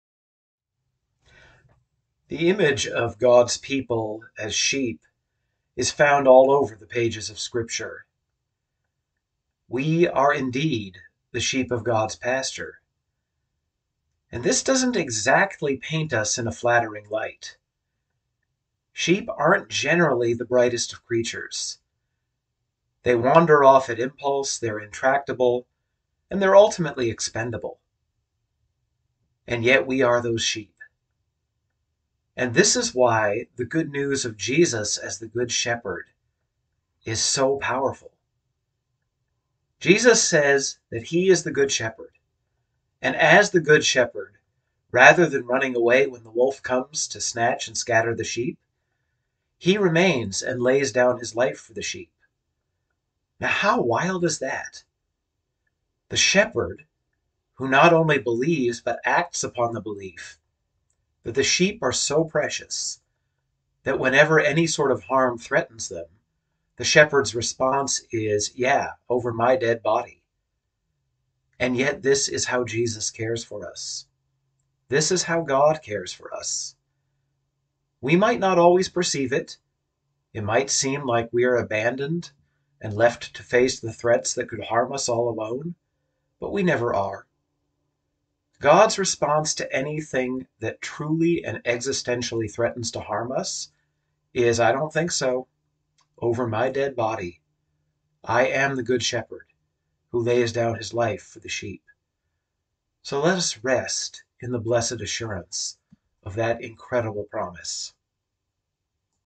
4 Easter 2024
Collect:  O God, whose Son Jesus is the good shepherd of your people: Grant that when we hear his voice we may know him who calls us each by name, and follow where he leads; who, with you and the Holy Spirit, lives and reigns, one God, forever and ever. Amen.

Scripture (John 10:11): Jesus said, “I am the good shepherd. The good shepherd lays down his life for the sheep."

Reflection:  Sheep are expendable.  They are bought and traded on the livestock market.  They are rented out to graze.  We keep and care for them for a season, but ultimately they become meat or just expire of natural causes, and we don't tend to be terribly moved.  But it's not so for Jesus.  For him the sheep are infinitely precious.  So precious in fact that his response to a perceived threat to them is, quite literally, "over my dead body!"  And we are those sheep.  This is the way God cares for us, both in this life and leading even more fully into the next.  We often may not feel or understand it, but God's answer to anything that threatens to truly harm us is "over my dead body!"  Let us rest in the blessed assurance of this wonderful promise.